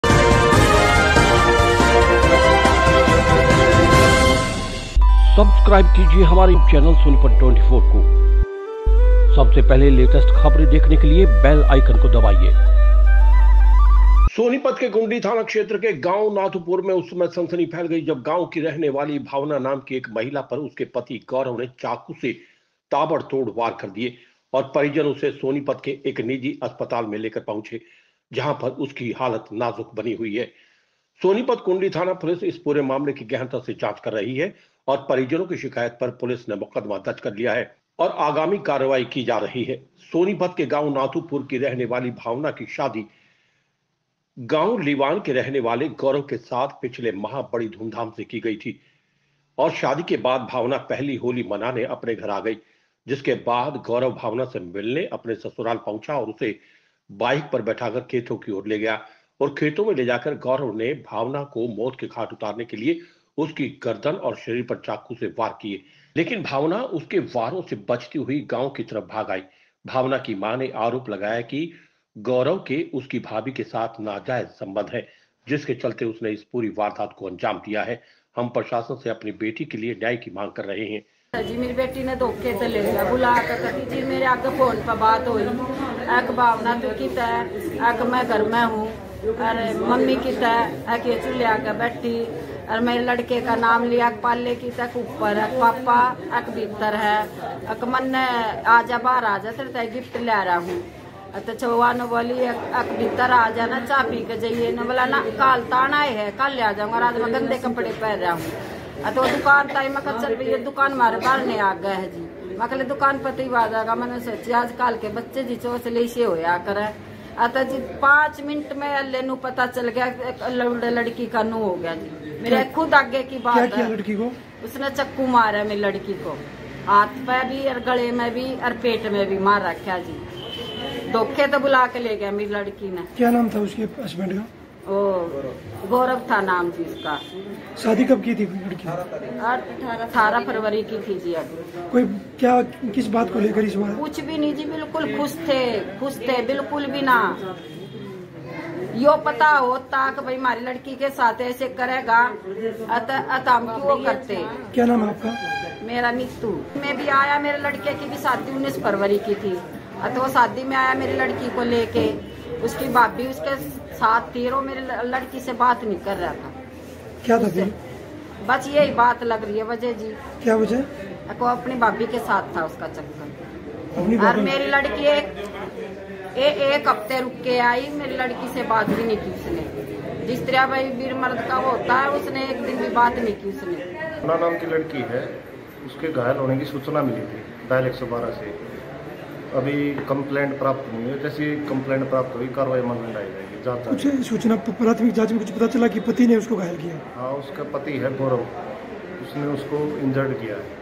पति पत्नी के रिश्ते को तार-तार कर देने वाला मामला आया सामने || Sonipat24
सोनीपत में एक बार फिर पति पत्नी के रिश्ते को तार-तार कर देने वाला मामला आया सामने
सोनीपत में पति ने पत्नी को मौत के घाट उतारने के लिए पत्नी के शरीर पर चाकू से वार
घायल पत्नी का सोनीपत के एक निजी अस्पताल में चल रहा है ईलाज
सोनीपत कुंडली थाना पुलिस कर रही है मामले की गहनता से जांच

 सोनीपत के कुंडली थाना क्षेत्र के गांव नाथूपुर में उस समय सनसनी फैल गई जब गांव की रहने वाली भावना नाम की एक महिला पर उसके पति गौरव ने चाकू से ताबड़तोड़ वार कर दिए और परिजन उसे सोनीपत के एक निजी अस्पताल में लेकर पहुंचे जहां पर उसकी हालत नाजुक बनी हुई है, सोनीपत कुंडली थाना पुलिस इस पूरे मामले की गहनता से जांच कर रही है और परिजनों की शिकायत पर पुलिस ने मुकदमा दर्ज कर लिया है और आगामी कार्रवाई की जा रही है।